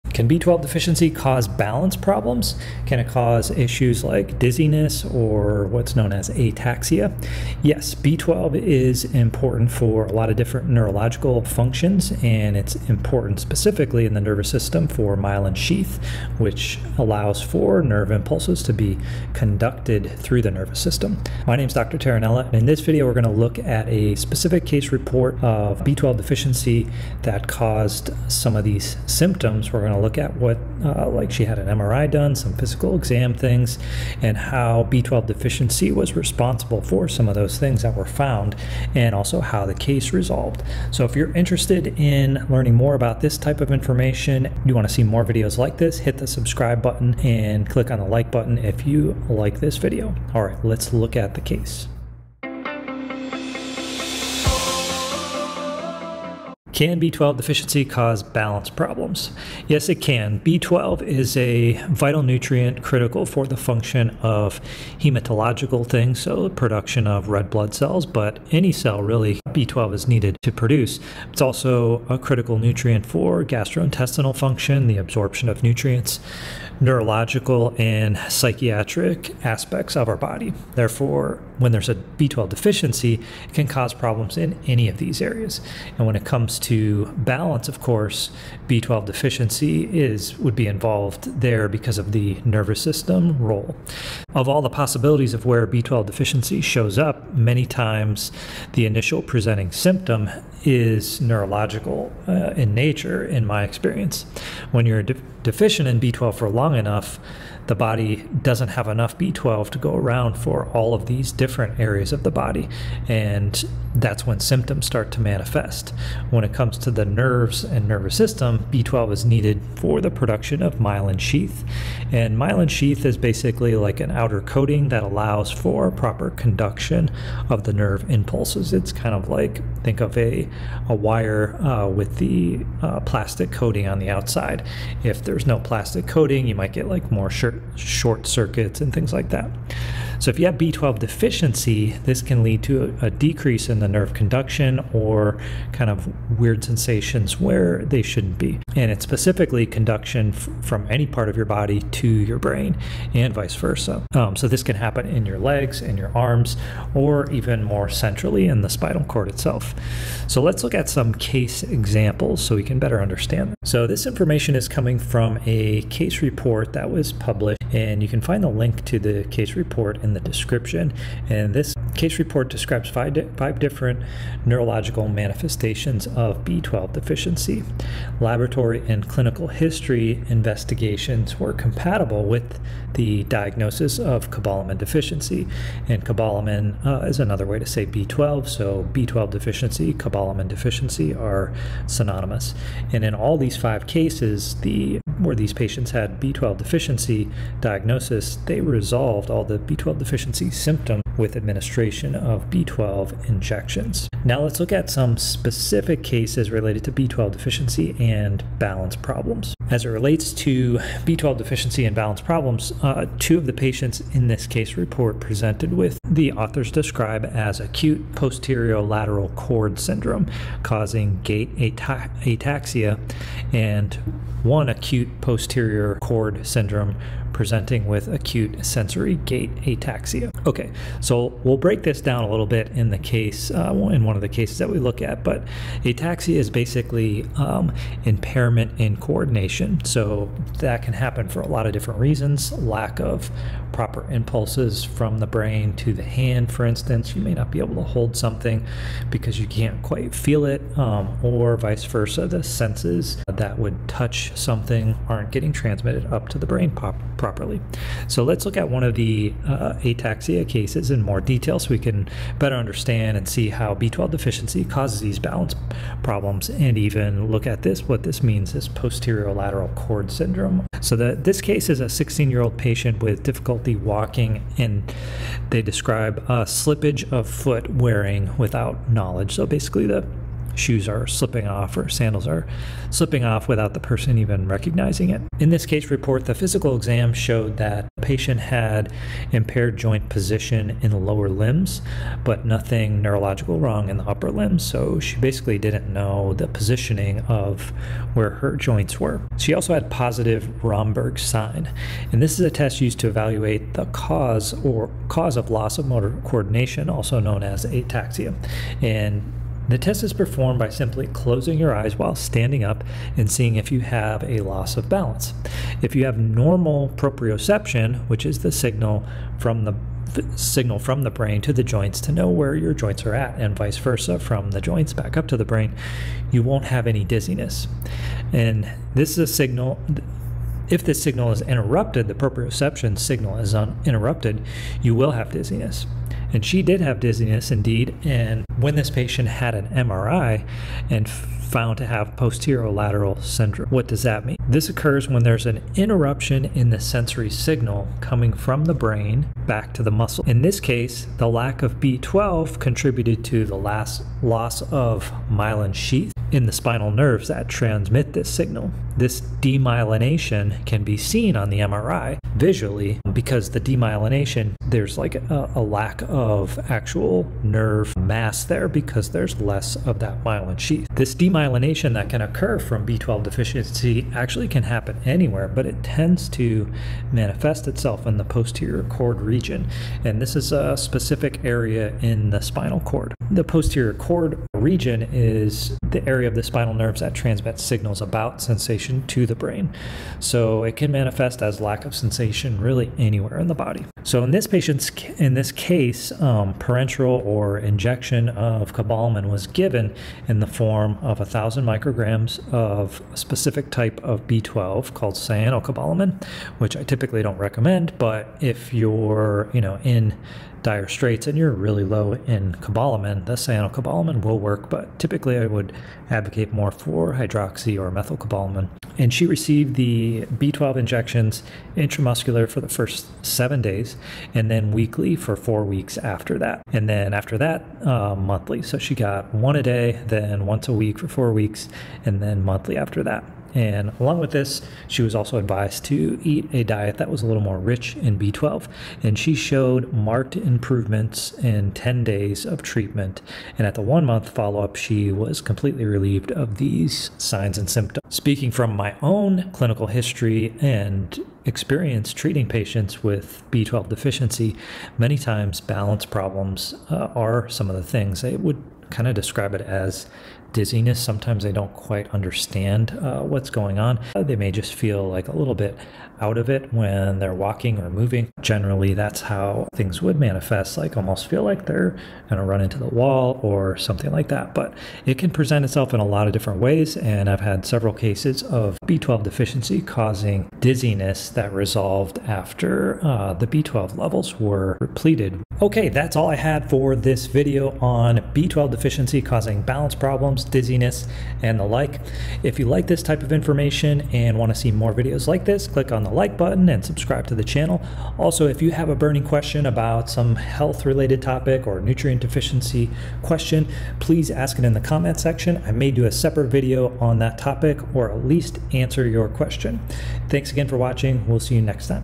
Can B12 Deficiency Cause Balance Problems?
Can B12 deficiency cause balance problems? Can it cause issues like dizziness or what's known as ataxia?  Yes B12 is important for a lot of different neurological functions.  It's important specifically in the nervous system for myelin sheath production which allows for nerve impulses to be conducted through out the nervous system.  In this article we will look at a specific case report of B12 deficiency that caused some of these symptoms.  We will also look at the findings on MRI and physical exam and how B12 deficiency was responsible for those findings.

Yes b12 deficiency can cause balance issues because of its role in the nervous system. B12 is a vital nutrient critical for many functions.  It is needed for the production of red blood cells but B12 is needed for production of any cell.  B12 is also a critical nutrient for gastrointestinal function and the absorption of nutrients.  It also plays a role in neurological and psychiatric aspects of our body.  Therefore when there is a B12 deficiency, it can cause problems in any of these areas.  When it comes to balance, of course, B12 deficiency is involved because of its role in the nervous system.

Of all the possibilities of where B12 deficiency shows up, many times the initial presenting symptom is neurological in nature.  When you are deficient in B12 for long enough the body doesn't have enough B12 to go around for all of these different functions in the body. This is when symptoms start to manifest.  When it comes to the nerves and nervous system, B12 is needed for the production of myelin sheath.  Myelin sheath is like an outer coating of the nerve that allows for proper conduction of the nerve impulses.  You can think of it like a wire with an outer plastic coating.  If there is no plastic coating, you might get more short circuits.  Similarly if you have B12 deficiency this can lead to a decrease in the nerve conduction, weird sensations, or sensations that are out of place.  The myelin allows for conduction of signals from any part of your body to your brain and vice versa.  So the odd sensations can happen in your legs and your arms or even more centrally in the spinal cord itself.  Let's look at some case examples so we can better understand how B12 deficiency balance problems might manifest. The information on this case of B12 deficiency and balance problems is coming from a case report that was published and you can find here.  This case report describes five different neurological manifestations of B12 deficiency.  In all five cases the laboratory, imaging, and clinical history were compatible with the diagnosis of cobalamin deficiency.  As a side note, cobalamin is another way to say B12. So B12 deficiency and cobalamin deficiency are synonymous.  In all five cases the B12 deficiency symptoms resolved with administration of B12 injections.  Now let's look at some specific cases related to B12 deficiency and balance problems.

Two of the patients in this case report presented with what the authors describe as posterior-lateral cord syndrome causing gait ataxia.  One of the two had a more acute case with  sensory gait.  First let's look at what is ataxia.  Ataxia is an impairment in coordination.  This impairment can happen for many reasons like a lack of proper impulses from the brain to the hand.  Imagine if you are not be able to hold a cup because you can't quite feel it.  In this case the nerves that transmit the feelings when you touch it aren't getting transmitted up to the brain.  This is similar to what can happen with B12 deficiency.  So let's look closer at one of these cases to better understand how B12 deficiency causes coordination and balance problems.

This case is a year old patient with difficulty walking.  The authors  describe a slippage of foot wearing without knowledge. The patient's shoes are slipping off without her  recognizing it.  Her physical exam also shows that the patient has impaired joint position in the lower limbs. She had nothing neurological wrong in the upper limbs.  So she didn't know the positioning of her joints in space.  She also had positive Romberg sign.  The Romberg test is used to evaluate the causes of motor coordination loss also known as ataxia